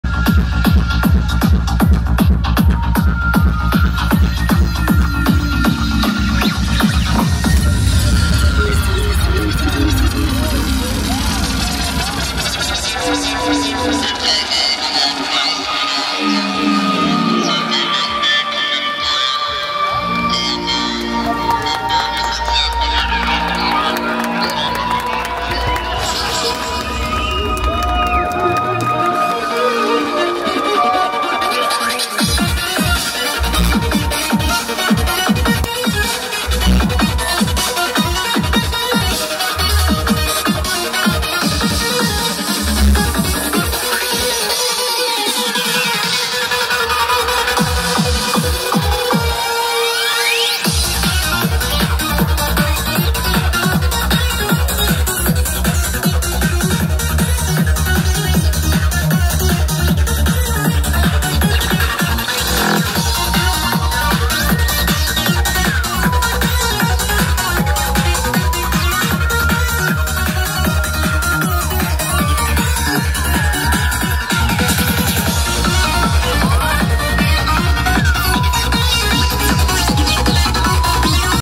Zna Gathering 2024 - Agneton blasting the dancefloor Crazy times - Portugal
If you have more info about this moment tell me in te comments ... Artist playing ? music name? etc...
More videos Comming soon...